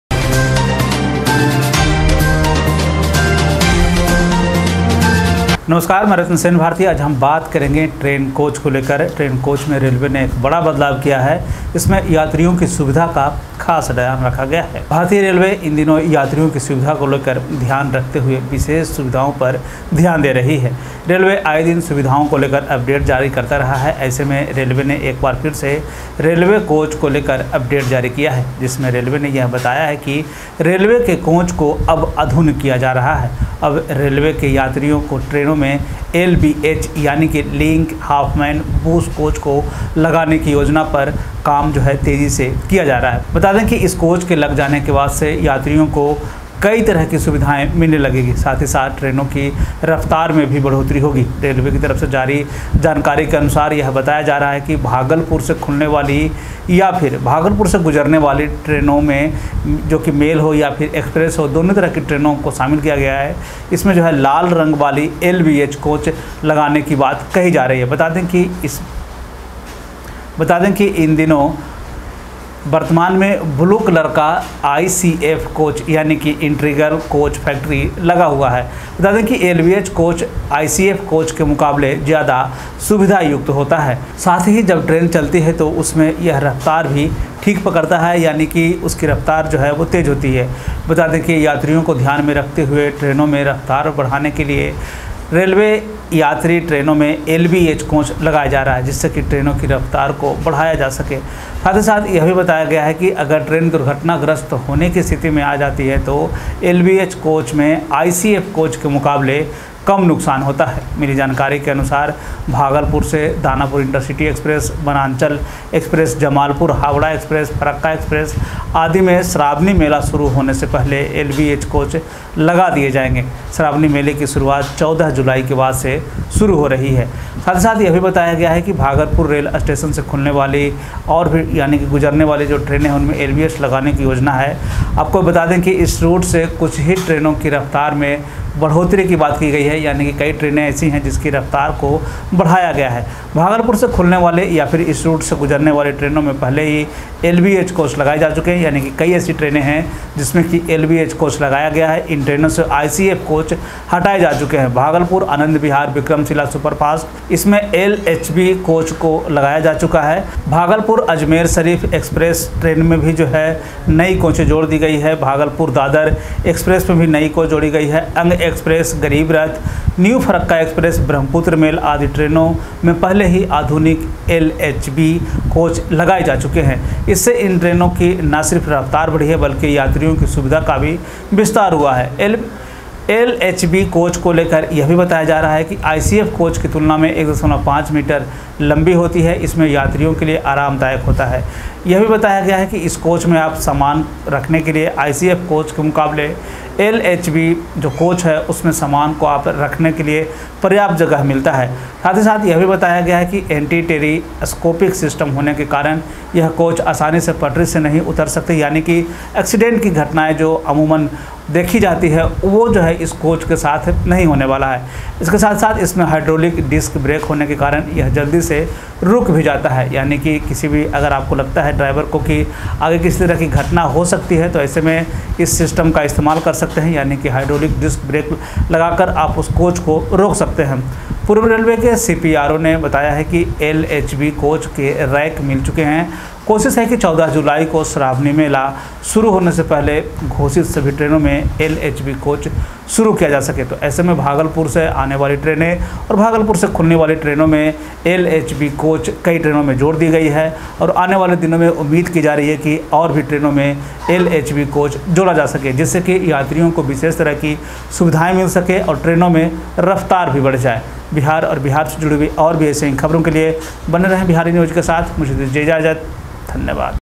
ट्रेन कोच में होगा बदलाव, यात्रियों को मिलेगी यह सुविधा
These days, keeping in mind the convenience of the passengers, Indian Railways is focusing on special facilities. Railways has been issuing updates about the facilities every day. In such a situation, the Railways has once again issued an update regarding the railway coach. In which the Railways has told that railway coaches are now being modernized. Now the plan to install LBH ie Link Hoffman Busch coach in the passenger trains of the Railways is going on at a brisk pace. Let us tell you that after the installation of this coach, the passengers will start getting the same facilities. Along with this, the speed of trains will also increase. According to the information released by the Railways, it is being told that the trains originating from Bhagalpur or passing through Bhagalpur, which include both mail and express trains, are being asked to install red colored LBH coaches.